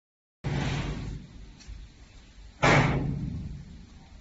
LGS frame loading today
The first container are beening loading today since the CNY holidays.
We are 100% back to normal now.